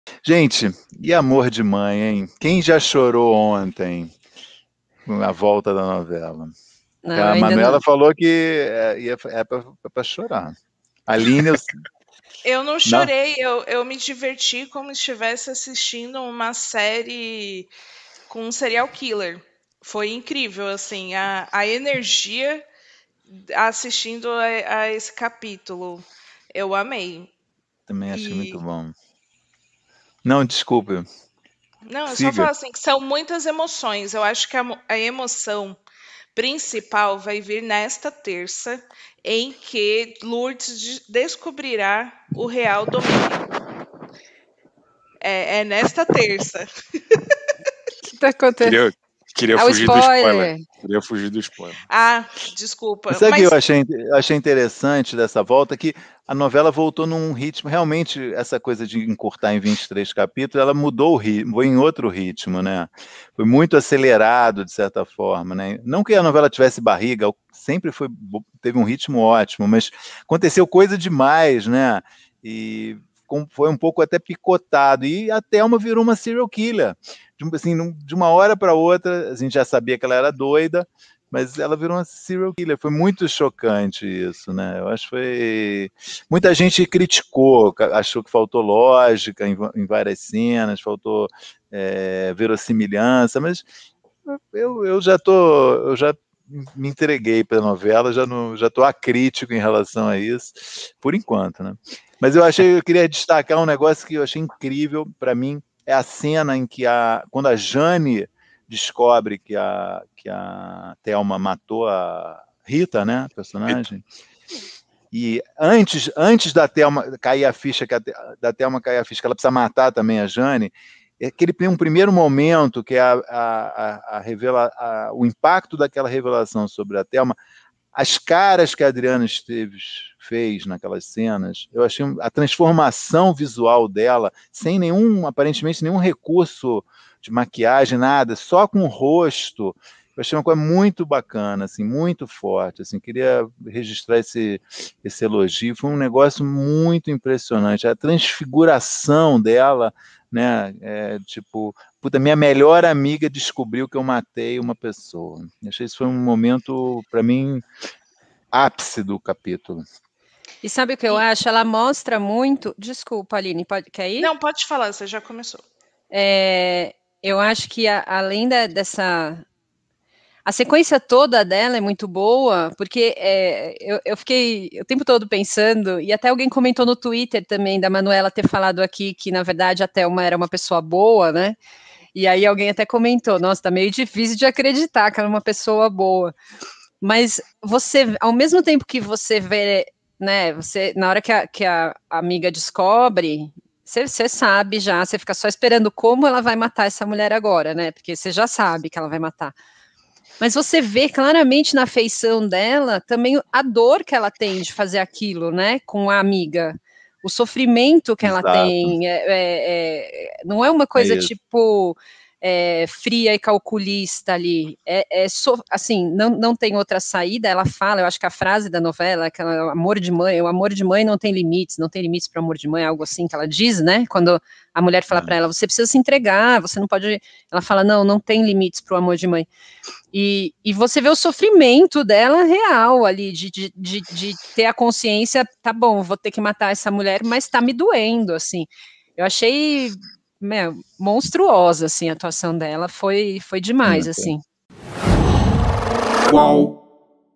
'Amor de Mãe': Thelma vira serial killer e ritmo da novela se acelera
No podcast UOL Vê TV #67 Aline Ramos, Chico Barney, Débora Miranda e Mauricio Stycer comentam o retorno da novela “Amor de Mãe”.